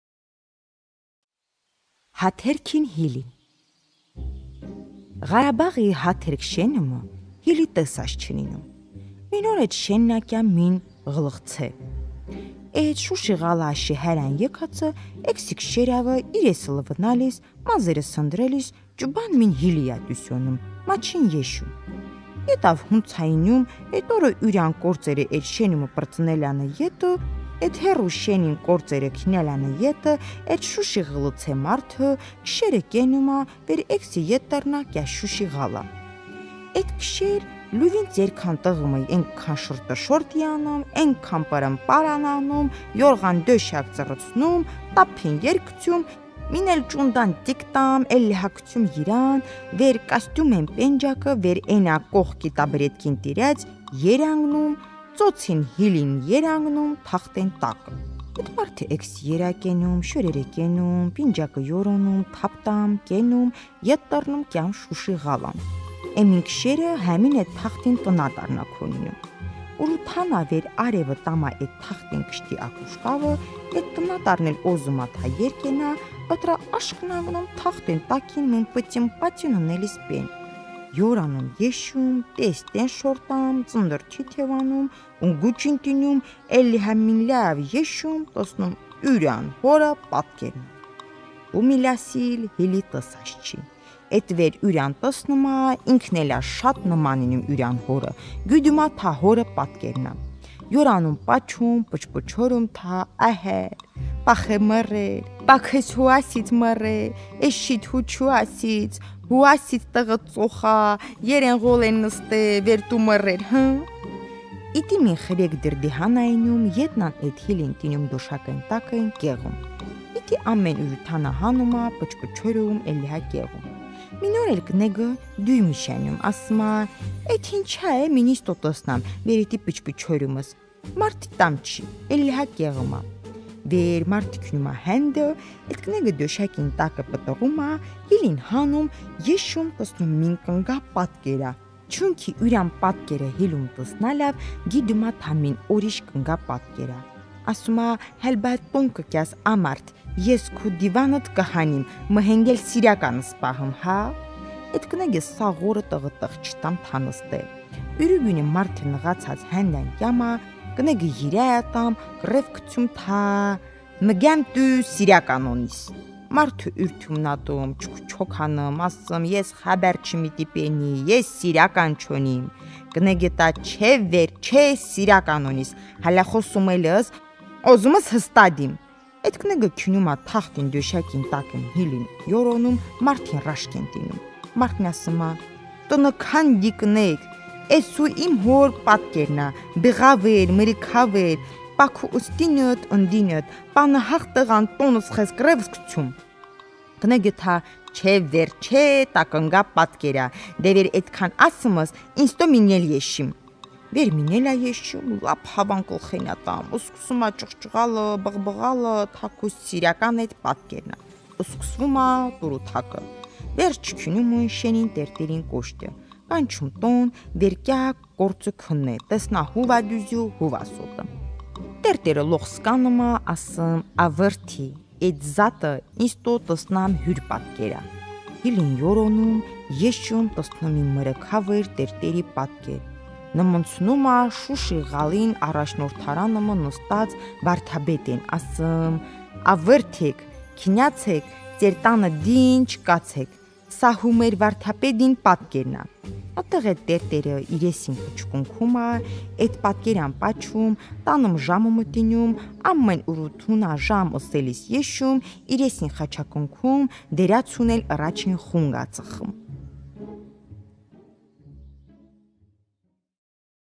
Հայ Ժողովրդական Հեքիաթներ Հաթերքին հիլին
Հատոր-V
Կարդում է Թամարա Պետրոսյանը
Հեքիաթը պատմել է Շամիր Ղազարյանը՝ Արցախի Մարտակերտի շրջանի Հաթերք գյուղից։
Գրի է առել Մ․ Գրիգորյանը 1950թ․